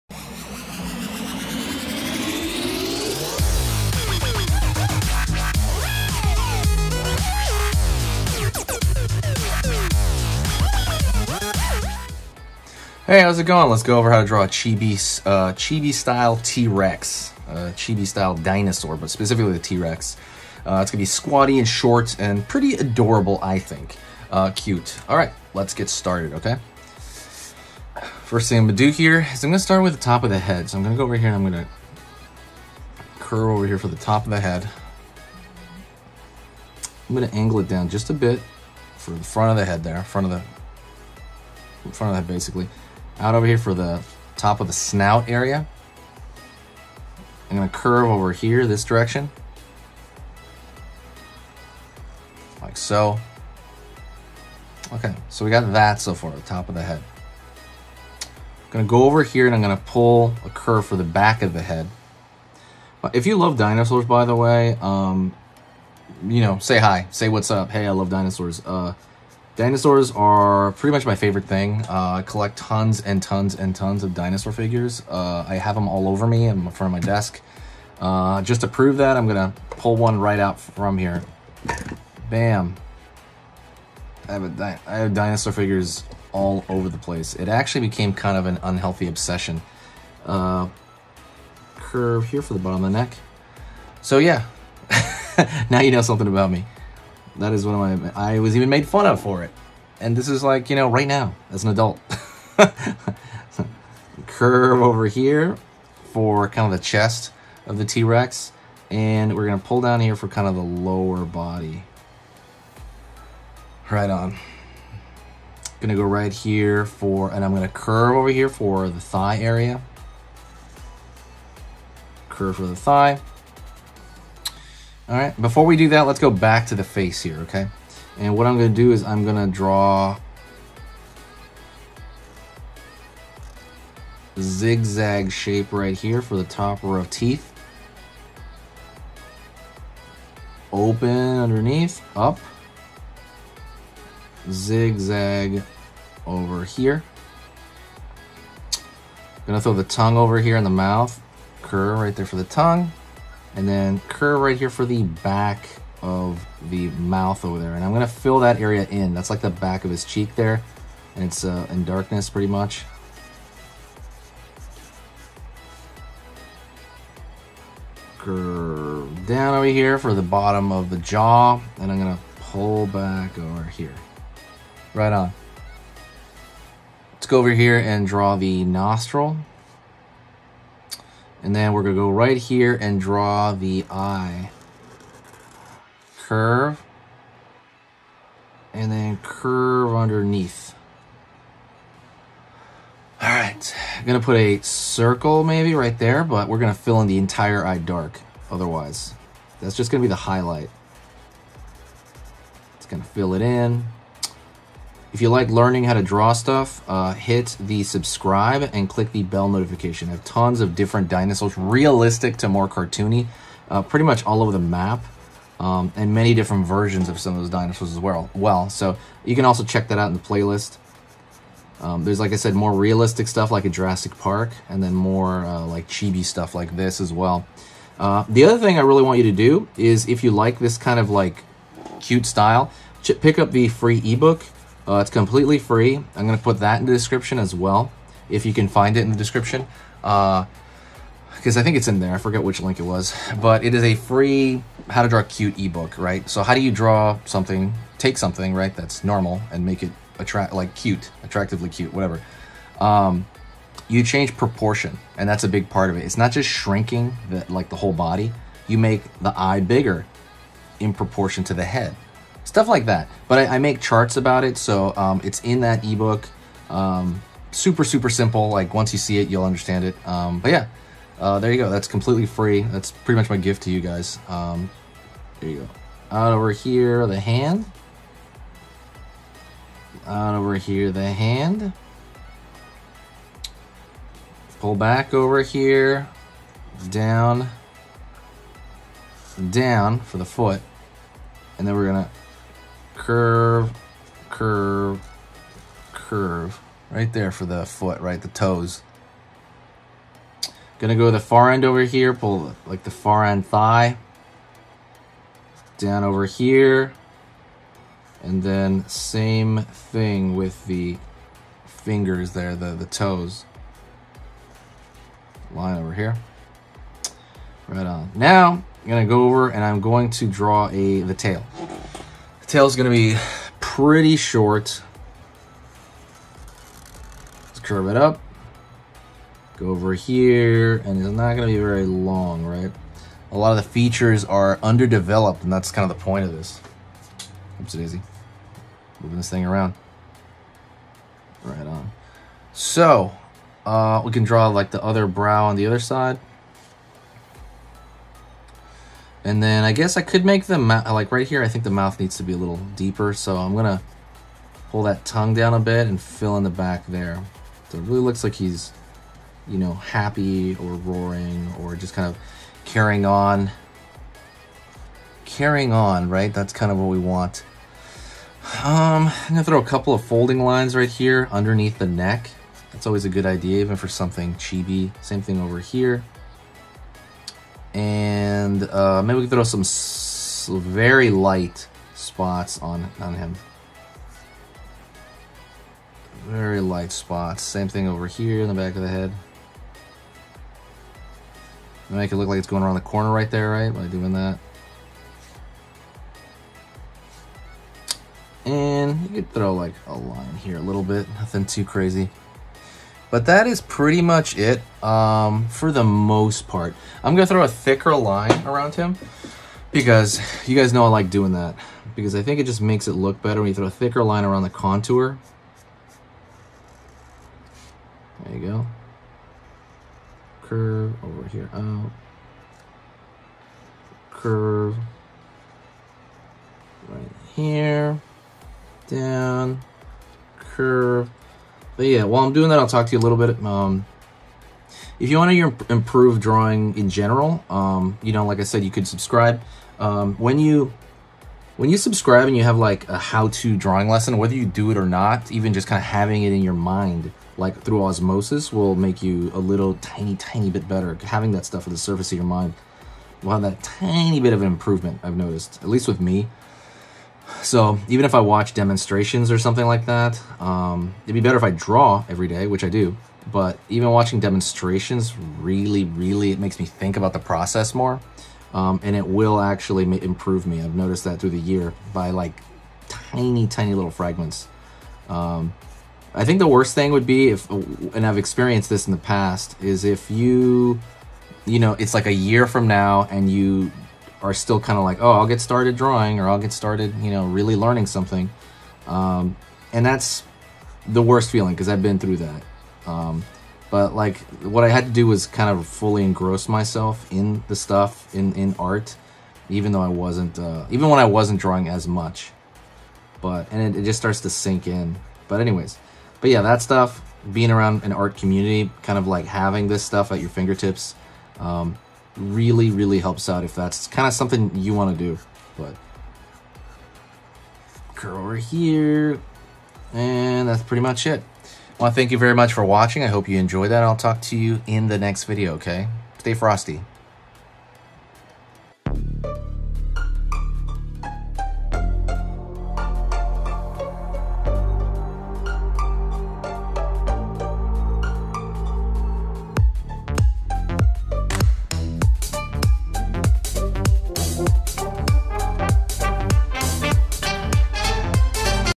EASY How to Draw BABY TREX DINOSAUR
Are you looking for an easy how-to guide on how to draw a baby trex dinosaur? This is a step-by-step drawing lesson. I hope you enjoy it.

Cool Links ___________________________________________________

Easy Tips on How to Draw DINOSAURS